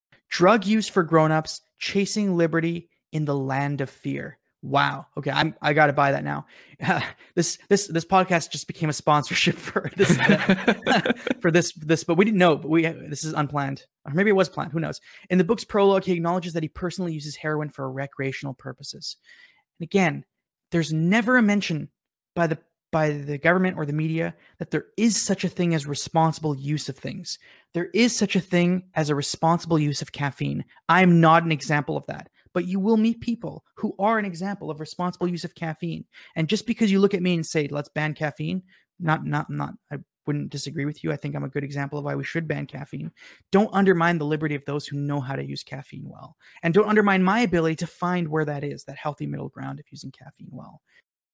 24.7 Responsible use of Caffeine || Aviv Milner
Aviv Milner is talking about the book Drug Use for Grownups, Chasing Liberty in the Land of Fear. Even heroin can be used in a responsible and recreational manner. No third party can prescribe what a “responsible” use of any substance is.